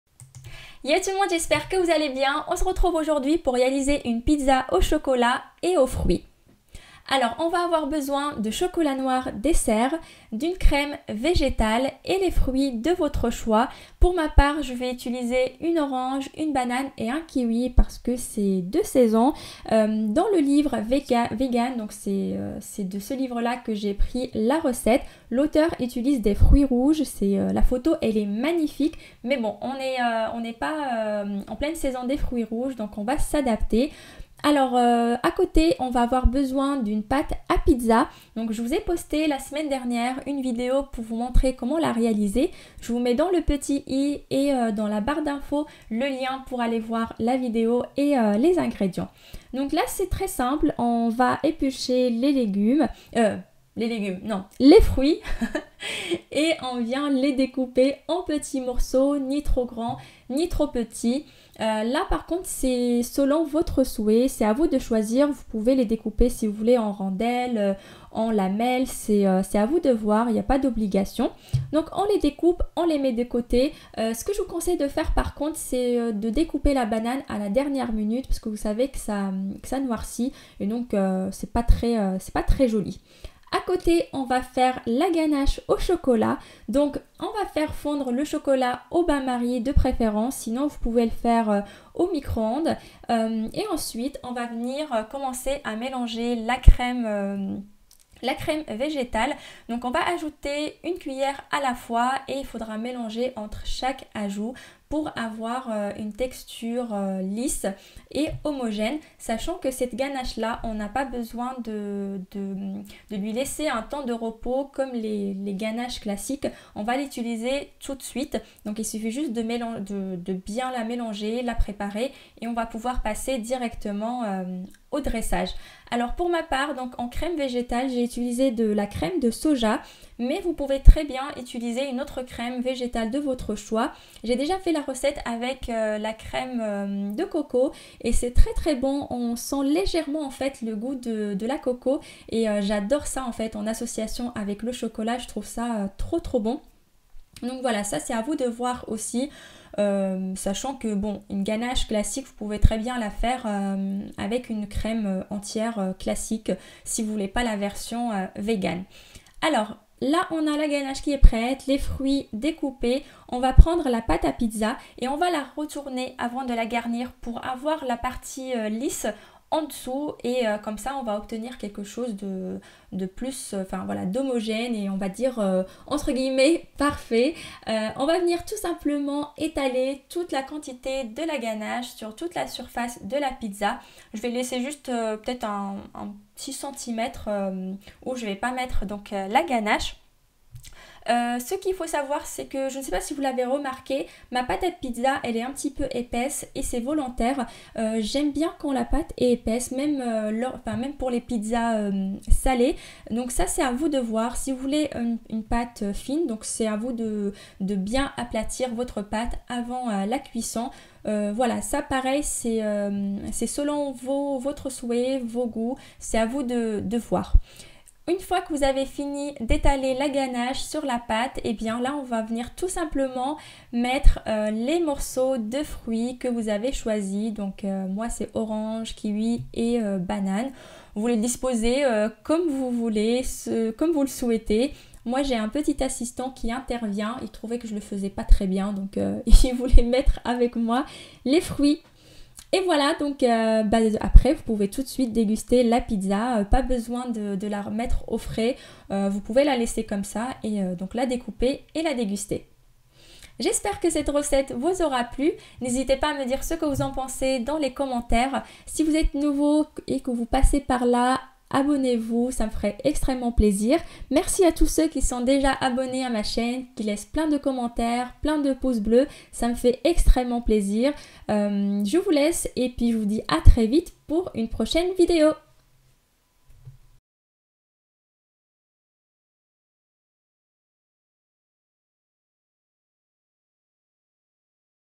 Pizza au chocolat Vegan
Après avoir réalisé la pâte à pizza maison, je vous propose de faire une pizza au chocolat. La recette est tirée du livre Vegan de Marie Laforêt. On va faire une ganache au chocolat vegan avec de la crème végétale. Pour ma part j'ai utilisé de la crème de soja mais vous pouvez utiliser celle de votre choix. Je vous conseille d'utiliser des fruits de saison et d'adapter les quantité en fonction de la taille de votre pizza.